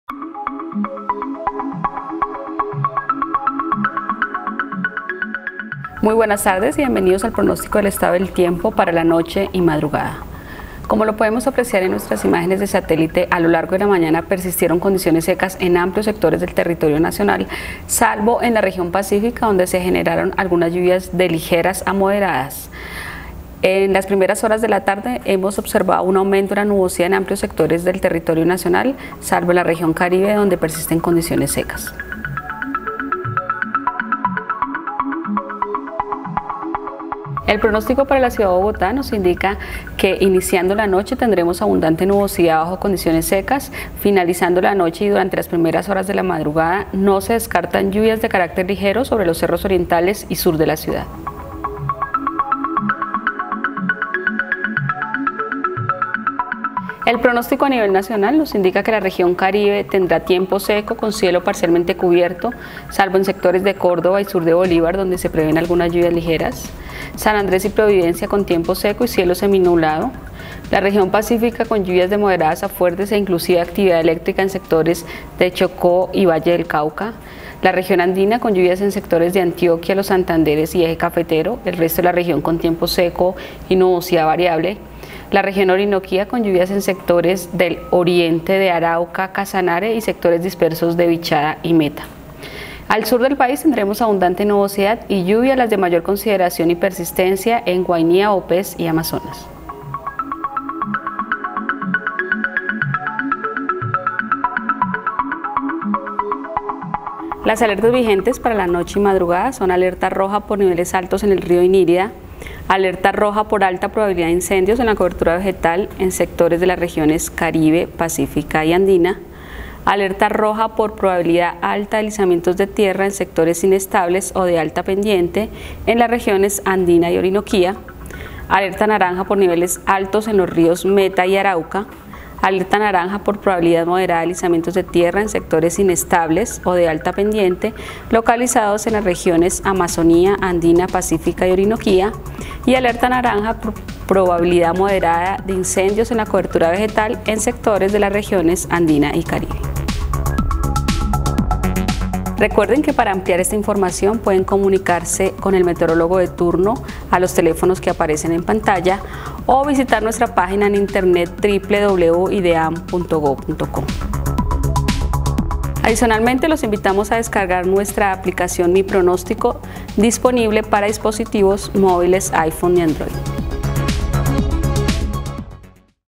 Pronóstico del Tiempo - Noche del Domingo 26 y Madrugada del Lunes 27 de Julio de 2015
Pronóstico del tiempo virtual para la Pronóstico del Tiempo - Noche del Domingo 26 y Madrugada del Lunes 27 de Julio de 2015 anunciando condiciones Hidrometeorológicas en Colombia.